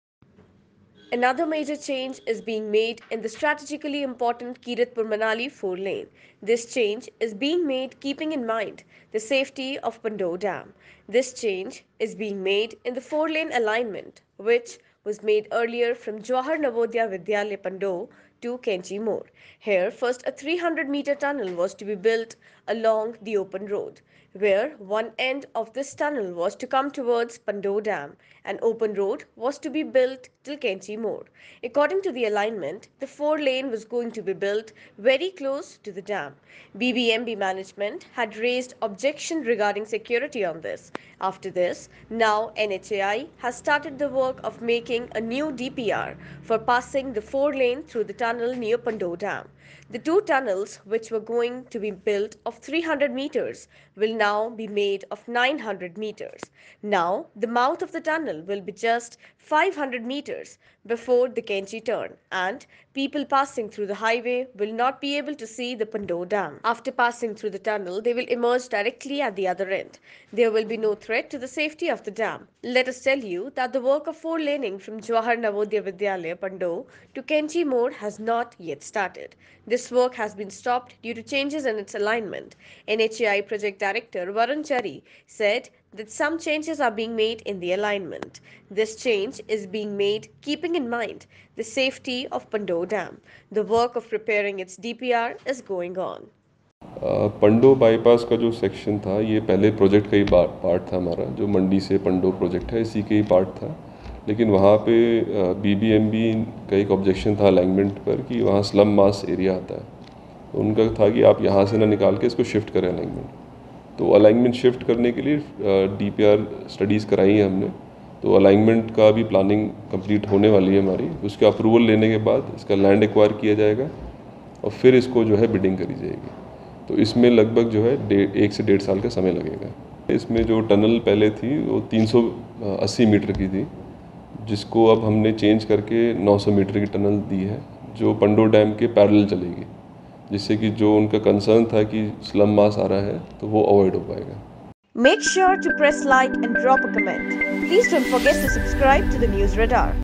💥After floods, Kiratpur-Manali NH's🛣️ to undergo alignment change💥
👉Significant alterations are underway for the Kitarpur-Manali four-lane project to ensure the safety of Pandoh Dam.

👉 The original alignment stretched from Jawahar Navodaya Vidyalaya Pandoh to Kainchi Mod and included a 300-meter tunnel alongside an open road, which raised safety concerns due to the proximity of the dam.